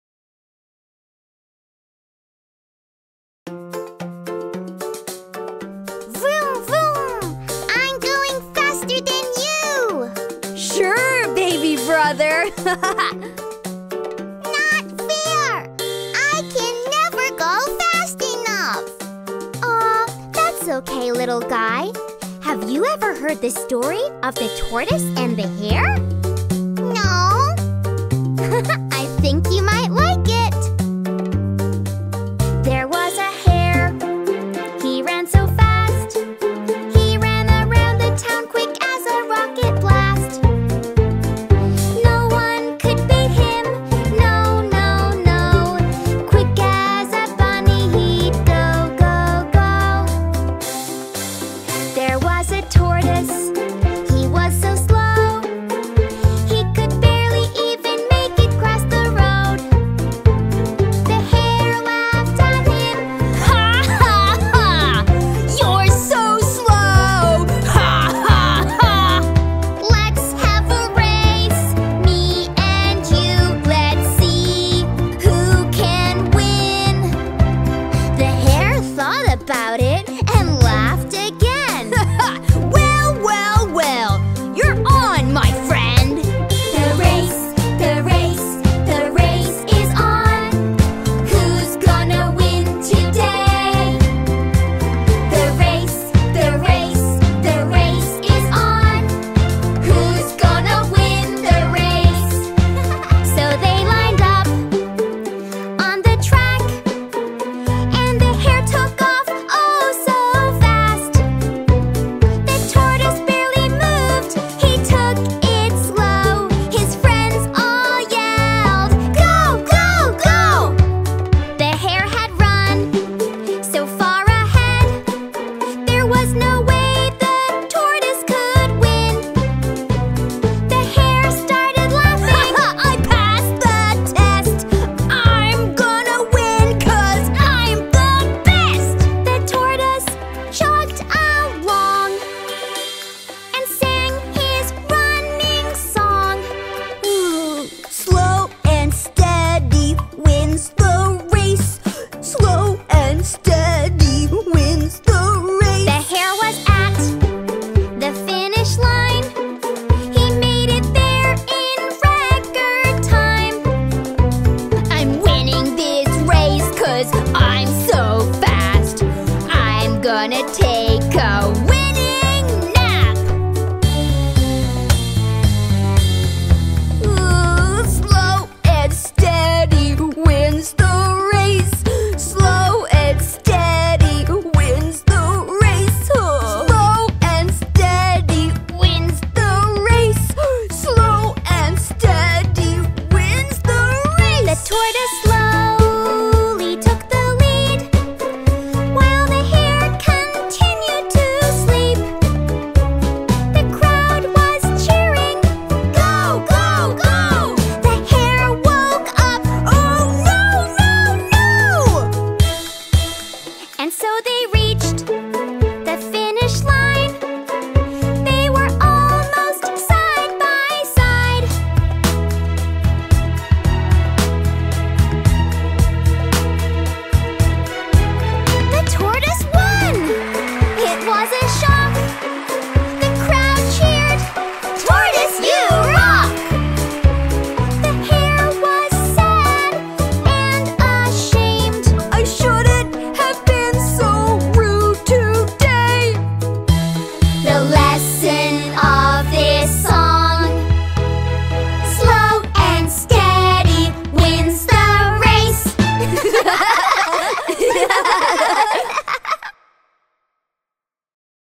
The Tortoise and The Hare Animated Stories for Kids by Kids Tv
Listening to stories is always a fun tour with Kids TV Fairytale and children's stories as it takes you to the world of best imaginations, be that a bedtime story, moral story, or any fairy tale that you all kids would want to hear during your playtime.